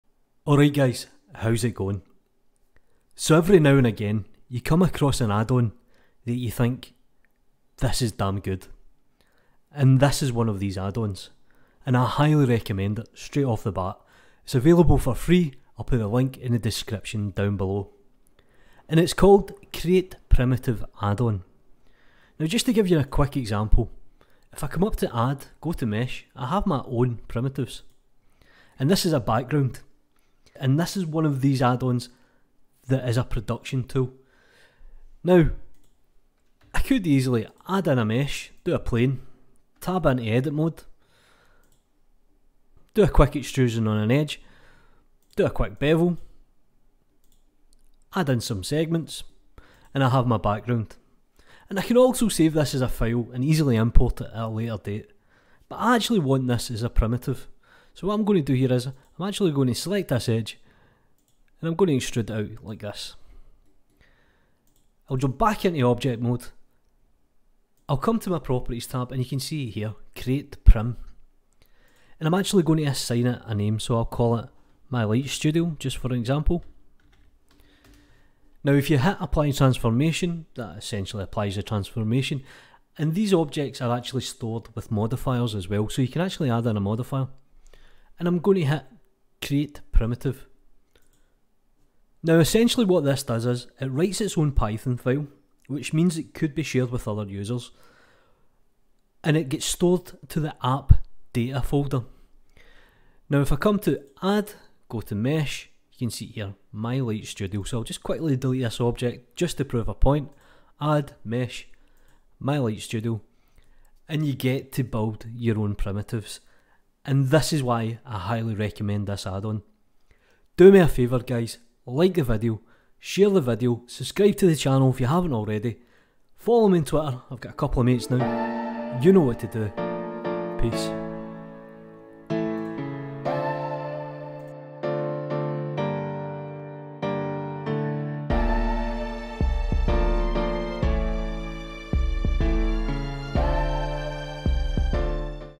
Blender 2.8 Add-on Overview: Create Prim addon - turns an object into add mesh addon (Free)
Recently updated to work with Blender 2.8 this addon lets you save an active object and export out a python file essentially creating an add mesh addon, no coding required.

... perfect for building up a collection of meshes that help speed up your workflow.